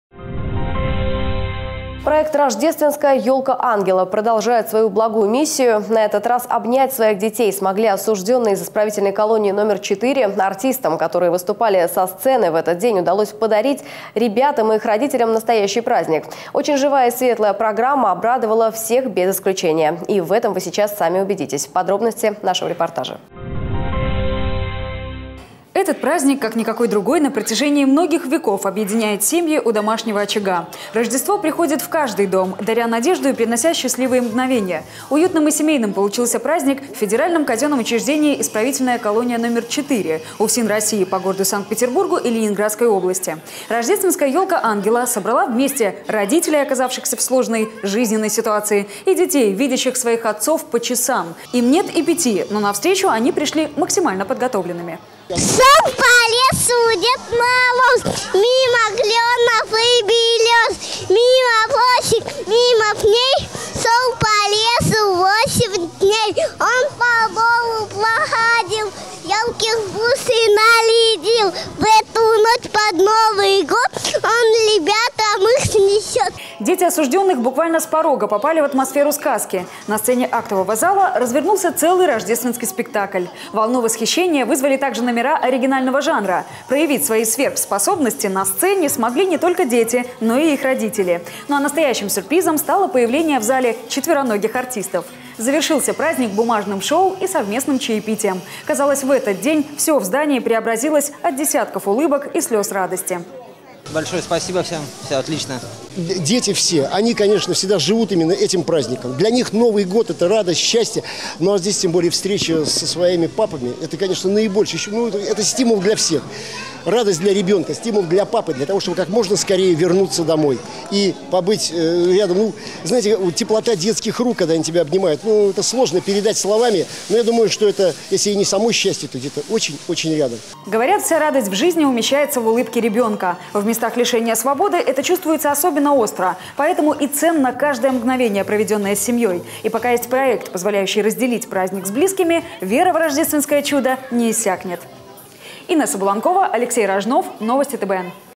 Мост между сердцами
Проект Рождественская елка Ангела продолжает свою благую миссию . На этот раз обнять своих детей смогли осужденные из исправительной колонии №4 . Артистам, которые выступали со сцены в этот день, удалось подарить ребятне и их родителям настоящий праздник. Очень живая и веселая программа обрадовала всех без исключения.